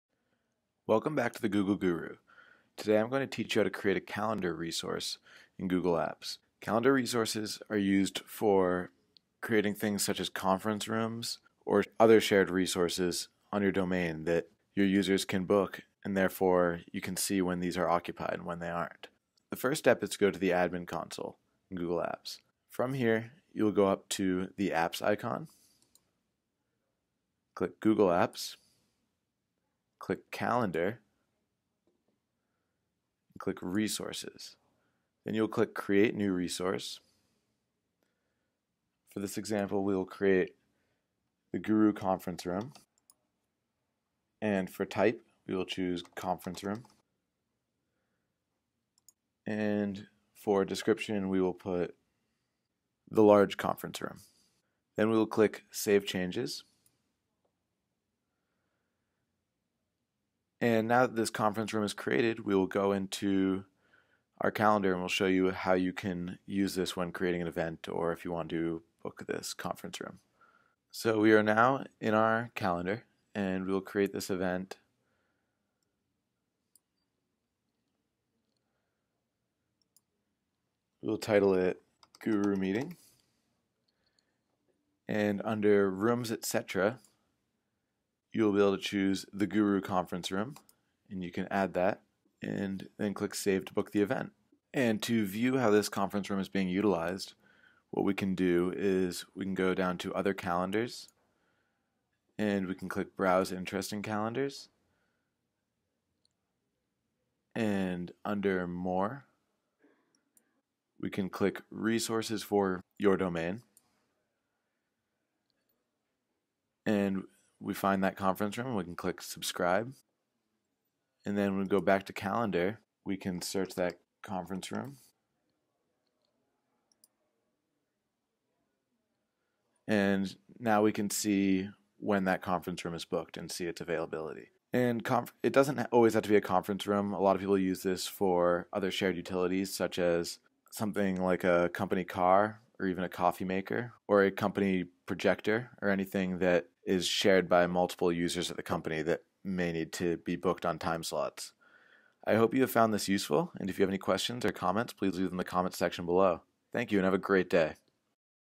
How To Create A Resource in Google Calendar
A calendar resource is a shared calendar that represents the usage of a physical object or space. This is commonly used for conference rooms, company vehicles, or even a shared tool such as an office coffee machine.

This video will walk you through the steps of creating and utilizing a calendar resource on your domain.